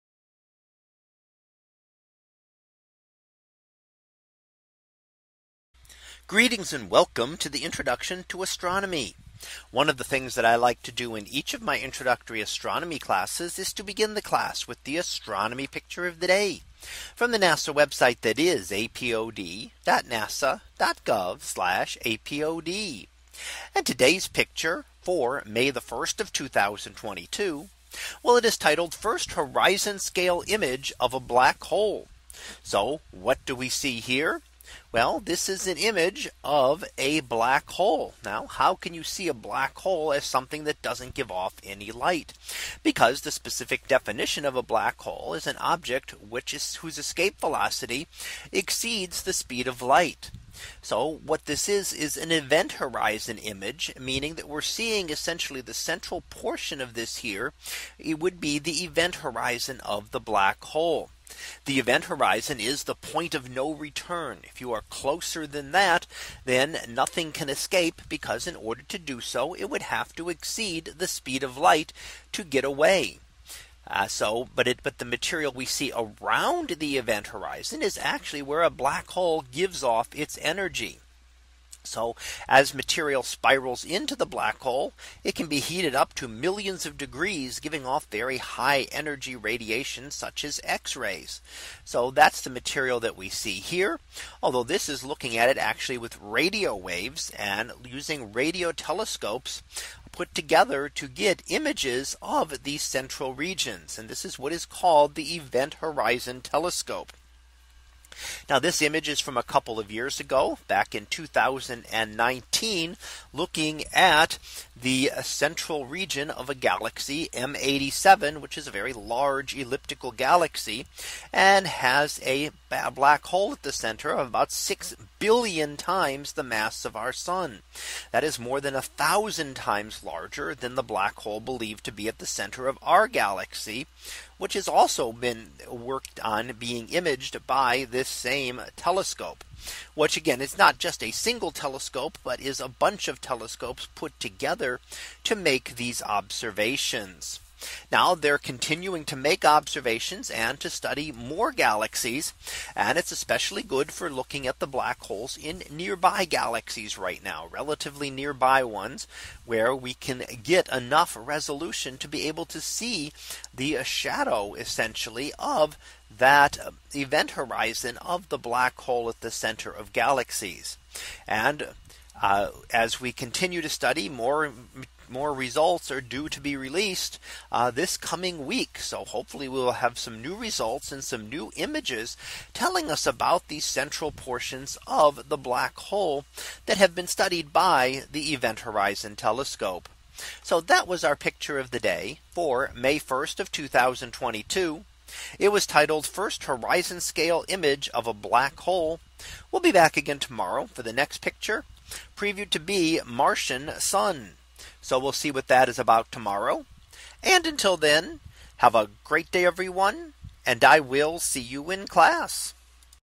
2022 May 01 - First Horizon Scale Image of a Black Hole
In today's image, we see an image of the black hole at the center of the galaxy known as M87. This is an image taken by the Event Horizon Telescope to look at the central regions of galaxies and to view the shadow of the black hole.